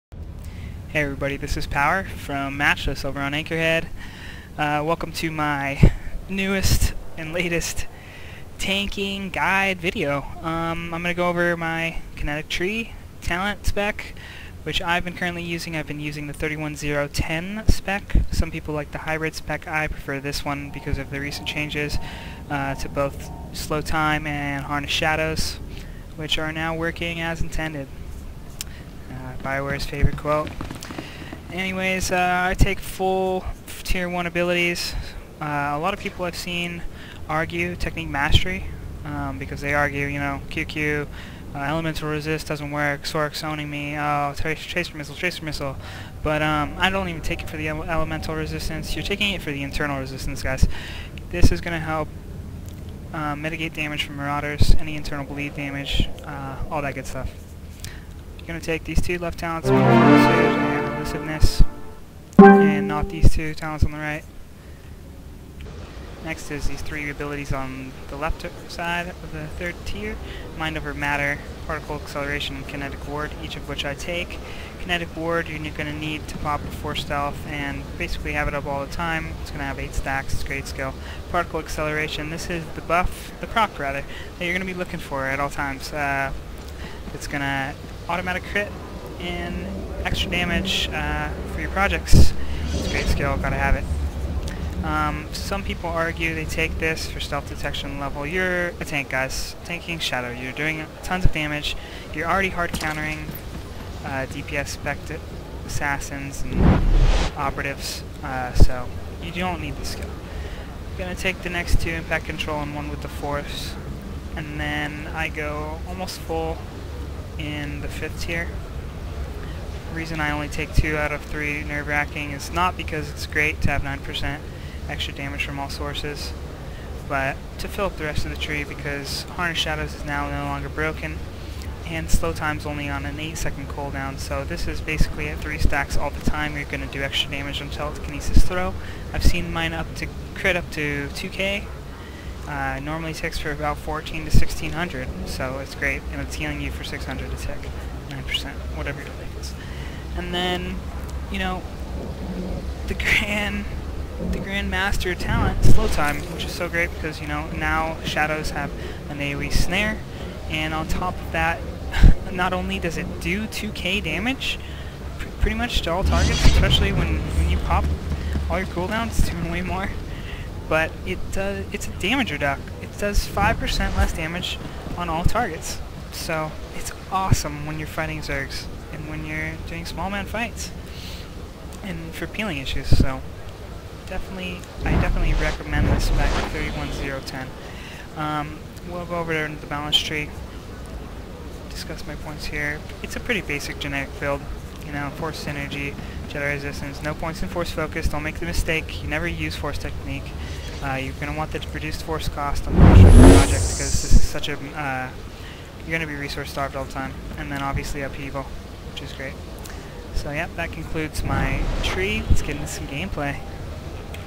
★ Powerr | Shadow Tank Spec
This video is me talking over the tanking pvp spec I have been using. This spec is 31 Kinetic 10 balance and specializes in Slow Time and Harnessed Shadows.  This is a great spec, tell me what you think!